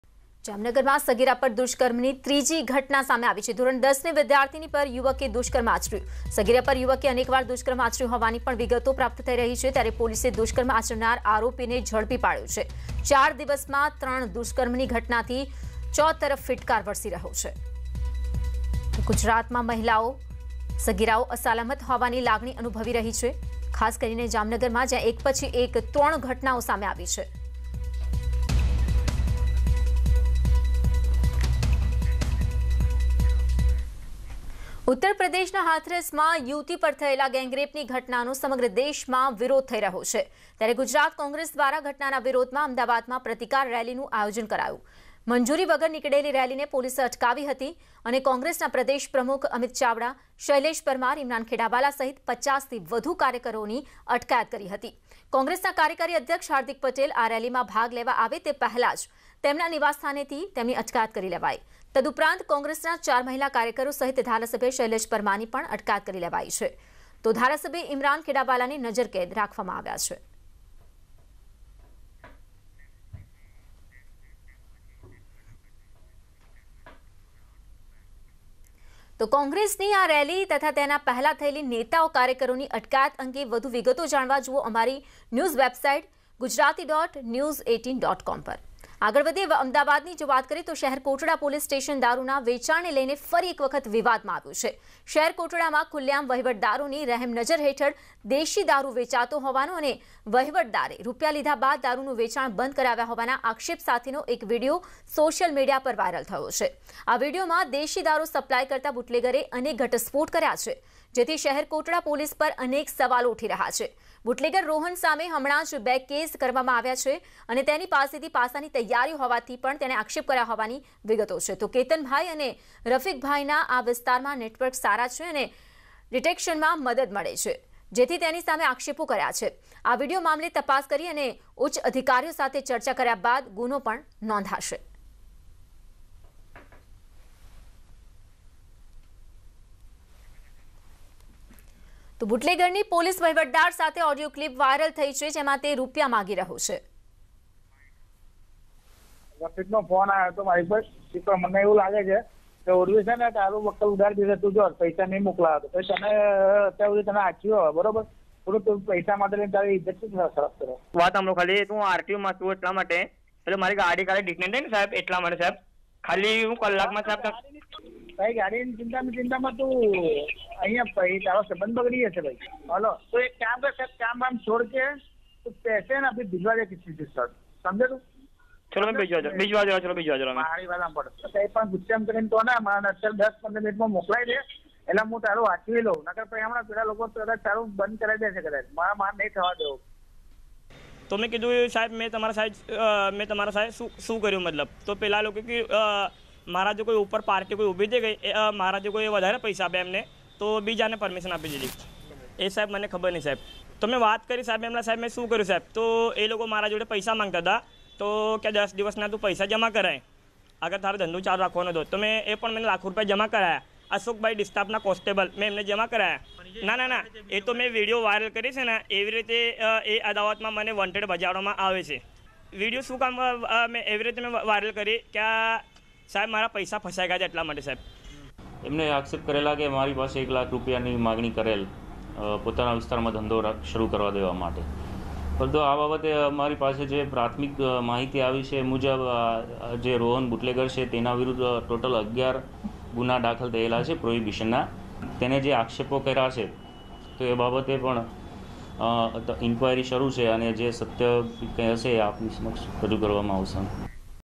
Gujaratનું 'હાથરસ' બનતું જામનગર! વધુ એક સગીરા પર દુષ્કર્મ, પીડિતાના પિતાએ આપઘાત કરતા થયો ખુલાસો
Gujaratનું 'હાથરસ' બનતું જામનગર! વધુ એક સગીરા પર દુષ્કર્મ, પીડિતાના પિતાએ આપઘાત કરતા થયો ખુલાસો. પીડિતાના પિતાએ ઝેરી દવા પીને આપઘાત કરતા ટૂંકી સારવાર દરમિયાન મોત નીપજ્યું હતું. જોકે, પોલીસ તપાસમાં જાણવા મળ્યું હતું કે તેની પુત્રી ઉપર દુષ્કર્મ થયું હતું. જામનગર શર્મસાર